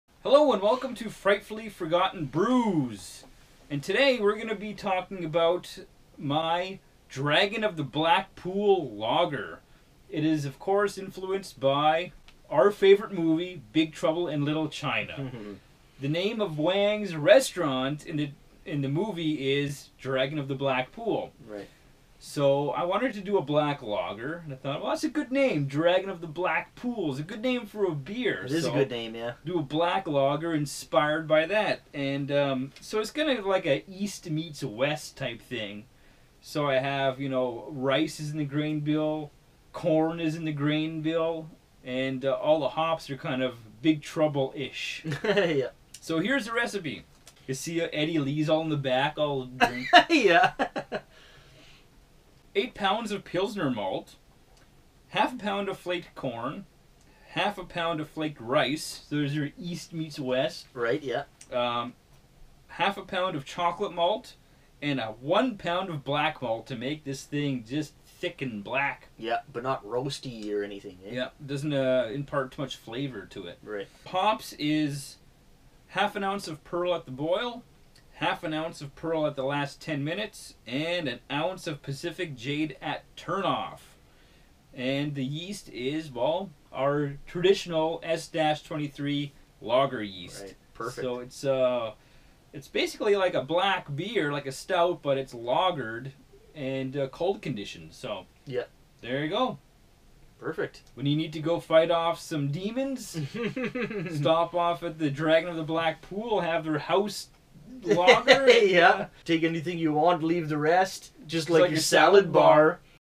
Dragon of the Black Pool Black Lager Recipe
Head on down to San Fransisco Chinatown, go to the Dragon of the Black Pool, and have the house black lager
23L Brew  - 5% - 22IBU
8lb Pilsner
.5lb Flaked Corn
.5 Chocolate Malt
.5lb Flaked Rice
1lb Black Malt

.5oz Perle @ Boil
.5oz Perle last 10min
1oz Pacific Jade @ Turn Off

Fermentis S-23 Yeast